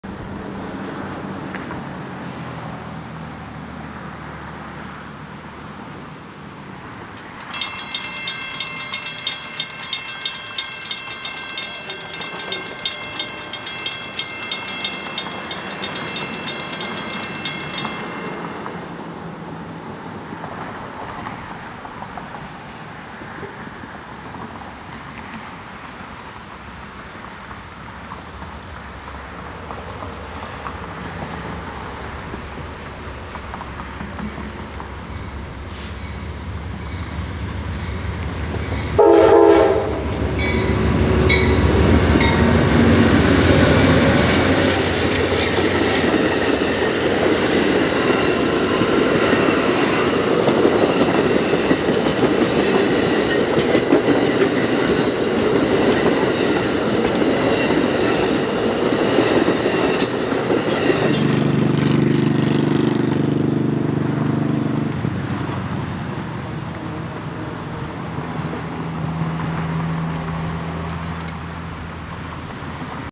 Amtrak Phase IV Heritage Unit Leads the Empire Builder
Amtrak's Phase IV heritage unit 184 leads the Empire Builder west, into Wauwatosa, Wisconsin on Canadian Pacific's Watertown Subdivision.  Wauwatosa is a quiet zone but the friendly engineer gives a couple blasts on the horn.